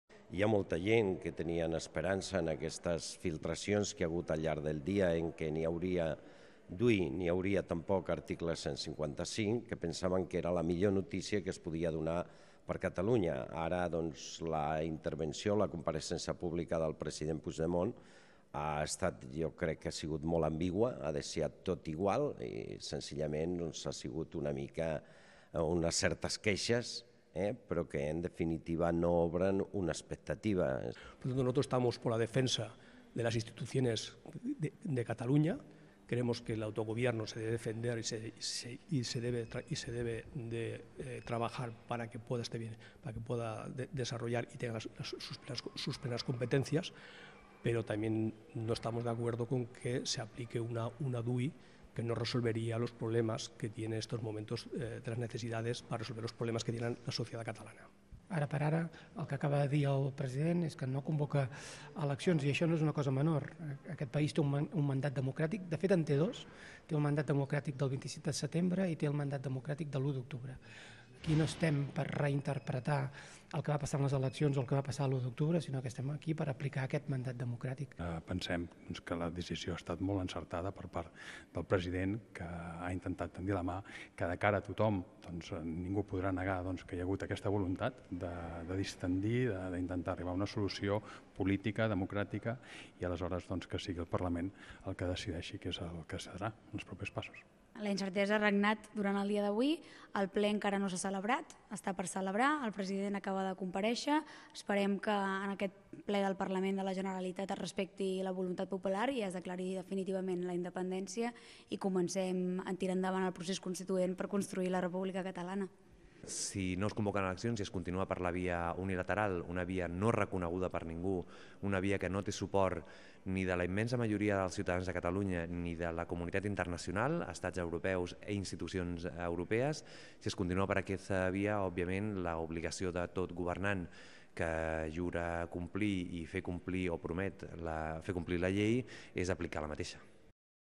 Els representants municipals opinen sobre la compareixença de Puigdemont
La compareixença del president Carles Puigdemont ha coincidit amb l’inici de la Comissió Informativa sobre les Ordenances Fiscals. Els representants municipals han pogut seguir la intervenció del president català des de la mateixa Sala de Govern i posteriorment han realitzat la seva pròpia valoració.